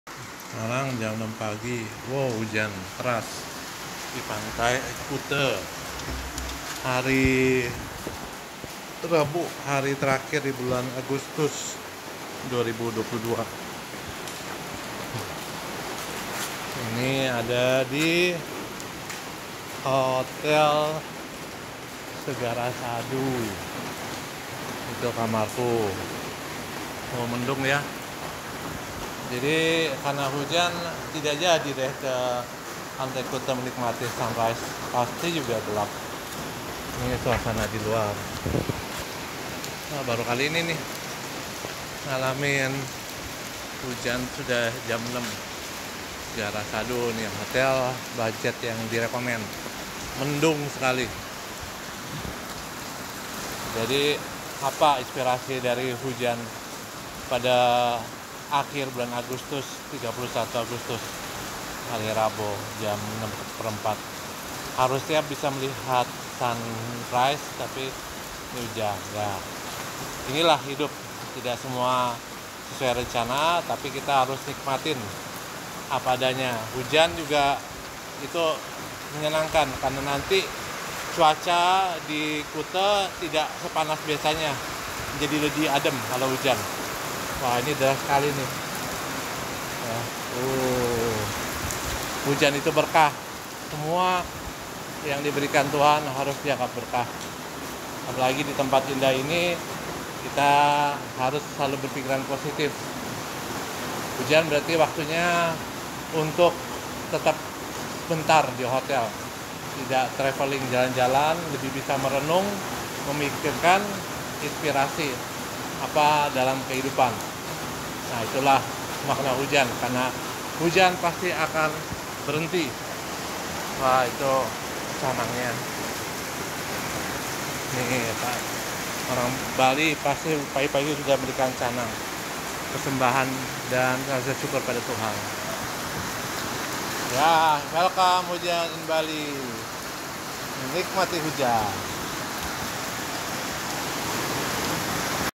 🌧 Hujan pagi di Kuta
Hujan pada akhir Agustus "bulan kemerdekaan' di Kuta, Bali... semoga membawa berkah

*Rabu, 31 Agustus 2022 jam 06.15 WITA saat sunrise @ Kuta, Bali